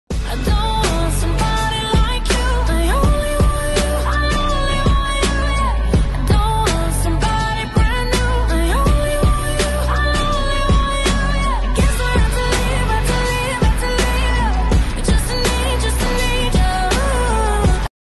The padlock hardware link chain pearl necklace
Sterling Silver material is my favorite. The adjustable chain has complete flexibility for you to layer it with your necklaces and bracelets.